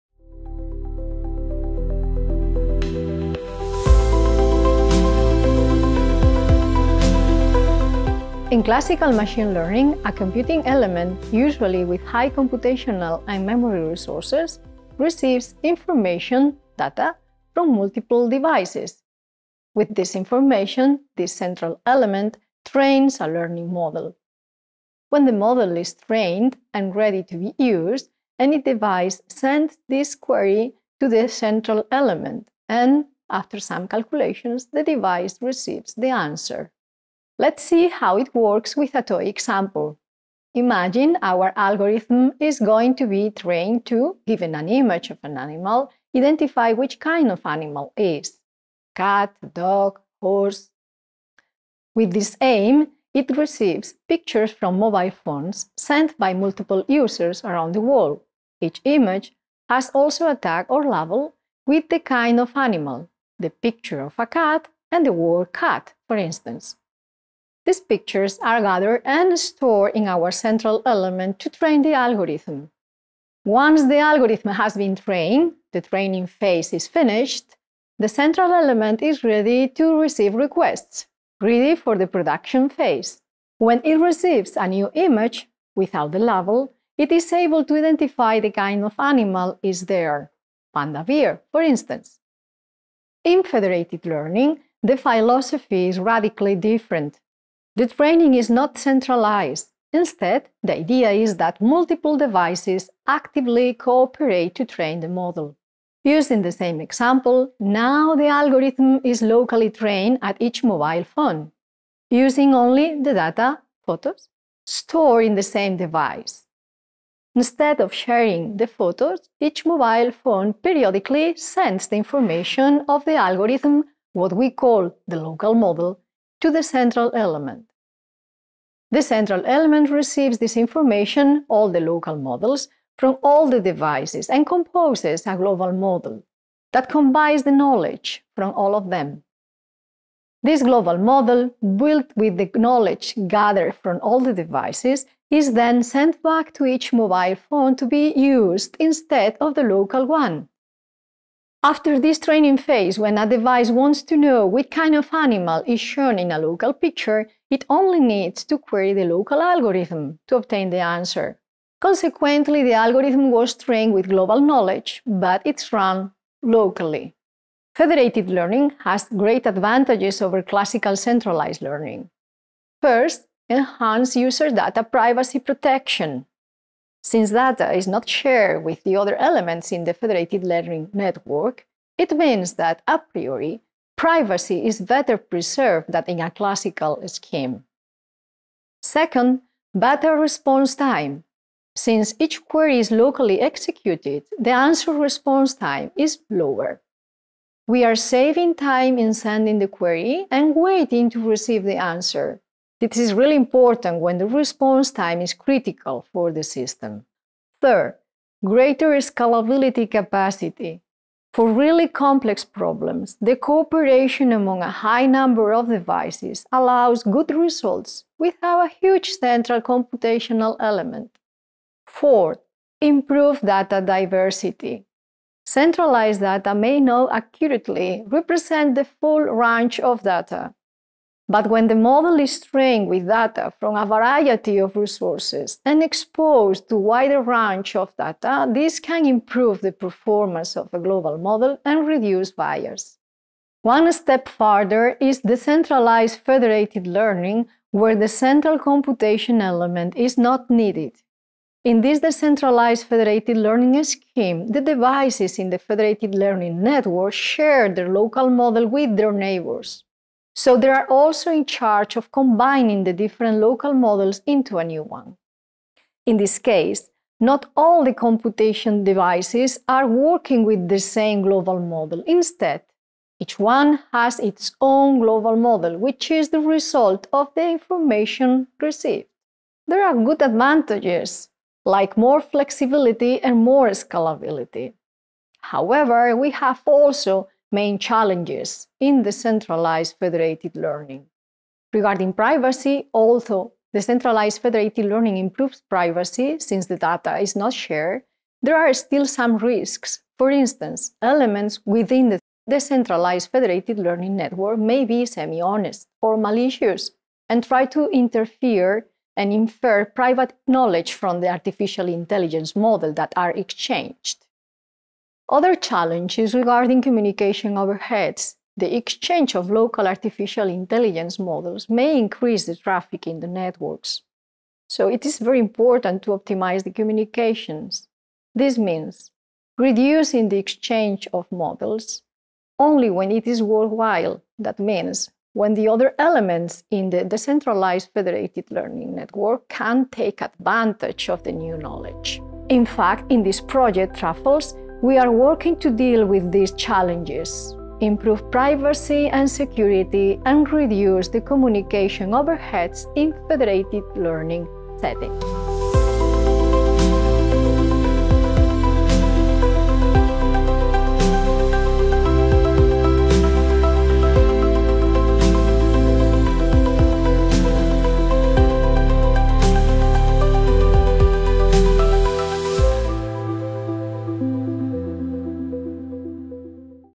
FL1: Introduction to Federated Learning
Federated Learning (FL) is a decentralized machine learning approach that addresses the limitations of traditional centralized learning. In this video, we explain with examples how federated learning works.